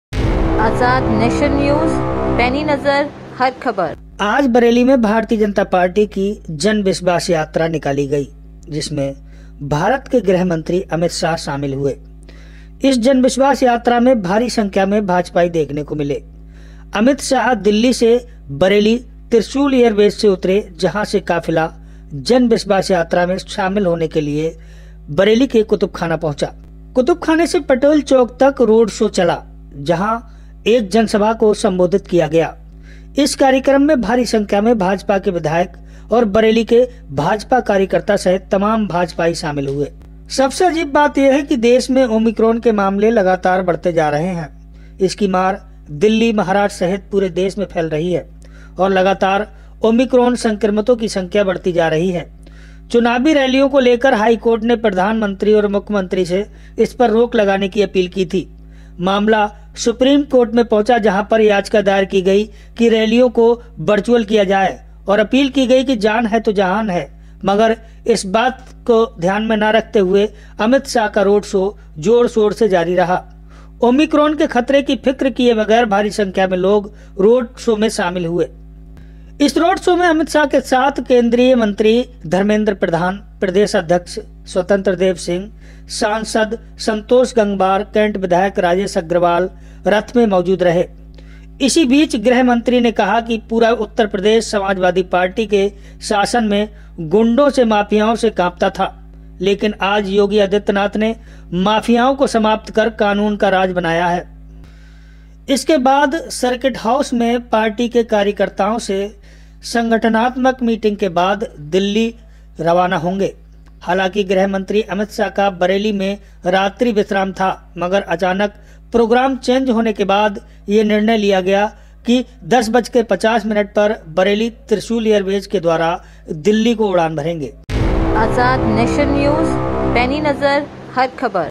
बढ़ते ओमीक्रोम के मामलों के बीच बरेली में अमित शाह की जन विश्वास यात्रा
Azad Nation News

बरेली - देश में बढ़ते ओमीक्रोन के मामलों के बीच बरेली में भारत के गृह मंत्री अमित शाह की जन विश्वास यात्रा निकाली गई जिसमें भारी संख्या में भारतीय जनता पार्टी के नेता और कार्यकर्ता मौजूद रहे.।